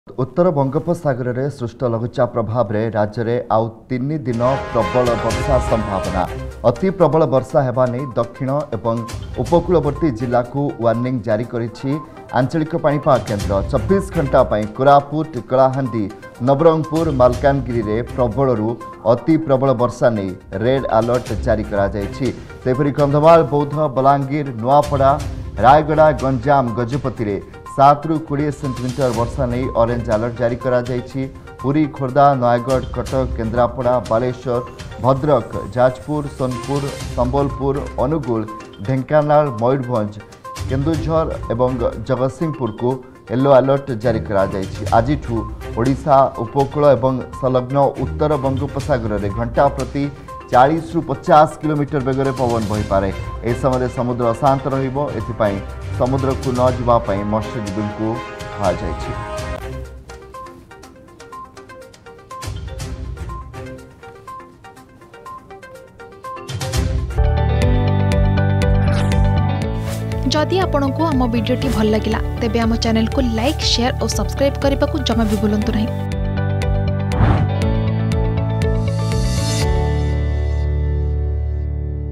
ରାଜ୍ୟରେ ଆଉ ୩ ଦିନ ପ୍ରବଳ ବର୍ଷା ସମ୍ଭାବନା || Weather Update
ଉତ୍ତର ବଙ୍ଗୋପସାଗରରେ ସୃଷ୍ଟ ଲଘୁଚାପ ପ୍ରଭାବରେ ରାଜ୍ୟରେ ଆଉ ୩ ଦିନ ପ୍ରବଳ ବର୍ଷା ସମ୍ଭାବନା । ଅତି ପ୍ରବଳ ବର୍ଷା ହେବା ନେଇ ଦକ୍ଷିଣ ଓ ଉପକୂଳବର୍ତ୍ତୀ ଜିଲ୍ଲାକୁ ୱାର୍ଣ୍ଣିଂ ଜାରି କରିଛି ପାଣିପାଗ କେନ୍ଦ୍ର । ୨୪ ଘଣ୍ଟା ପାଇଁ କୋରାପୁଟ, କଳାହାଣ୍ଡି, ନବରଙ୍ଗପୁର, ମାଲକାନାଗିରିରେ ପ୍ରବଳରୁ ଅତି ପ୍ରବଳ ବର୍ଷା ନେଇ ରେଡ୍ ଆଲର୍ଟ ଜାରି ରହିଛି ।